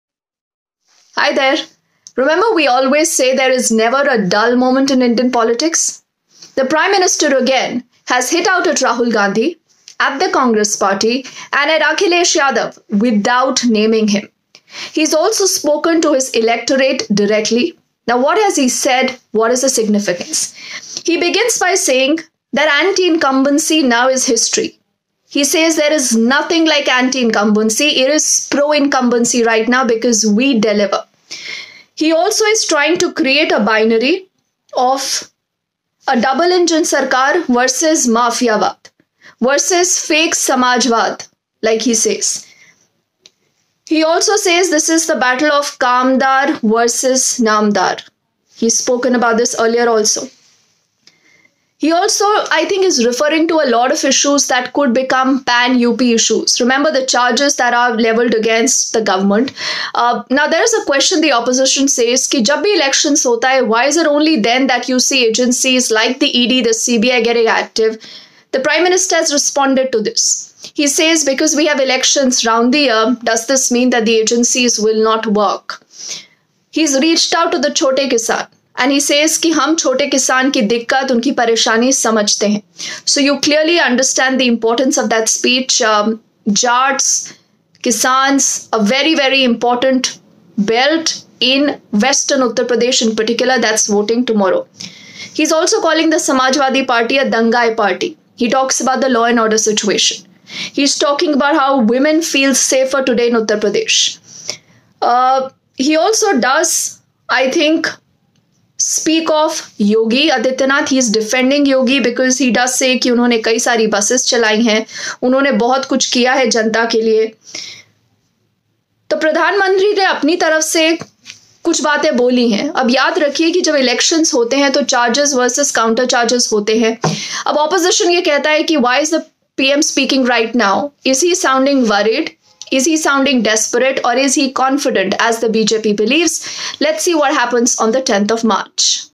Is BJP confident or looking nervous?
Did you hear PM @narendramodi?

BJP sounding confident?

Or @yadavakhilesh getting under their skin?